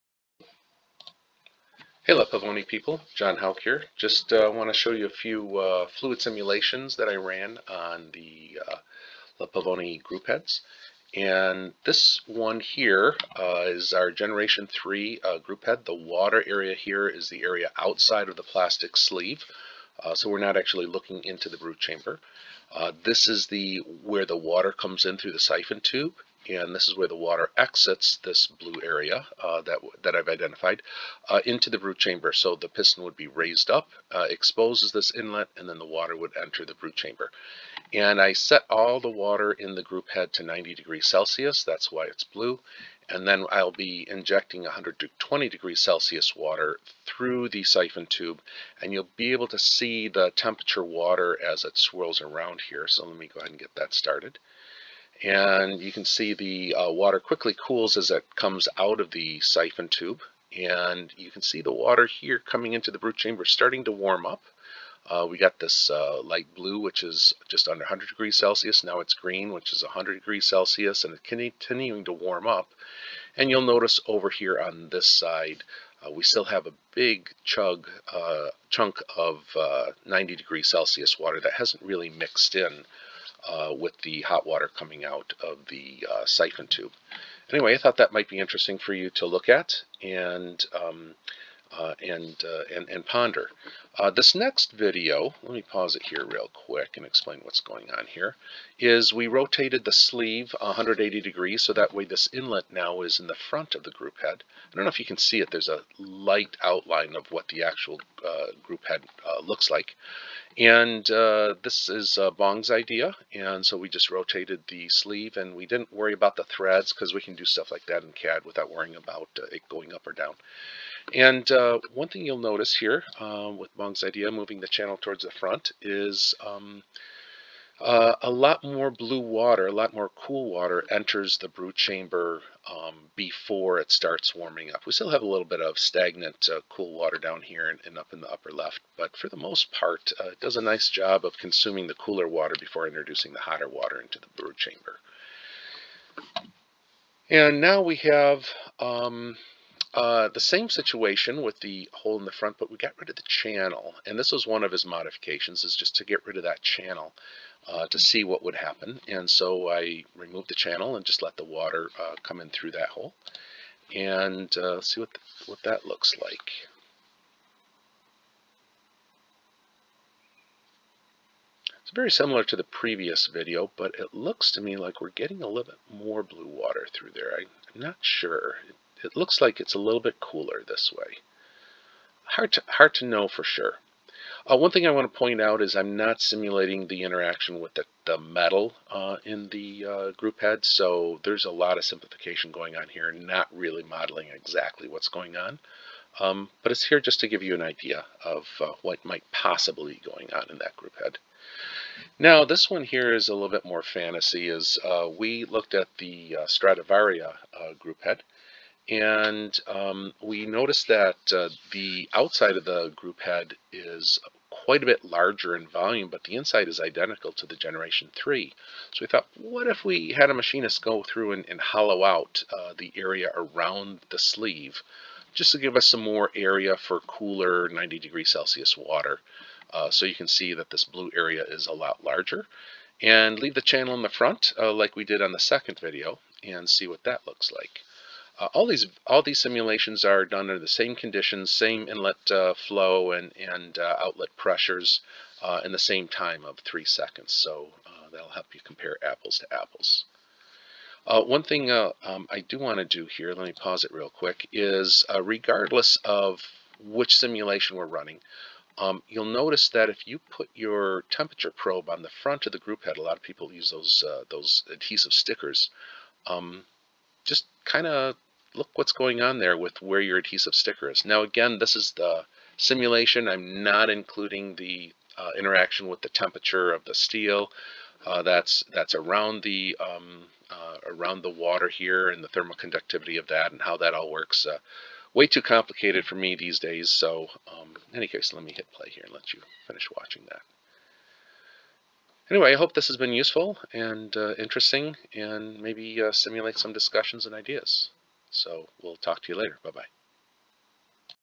13. Flow & Temperature Simulation - La Pavoni Lever Espresso Machine
An exploration of the OnShape CAD model of the La Pavoni Espresso Machines to see how the water swirls around the group head sleeve and the effect on temperature entering the brew chamber.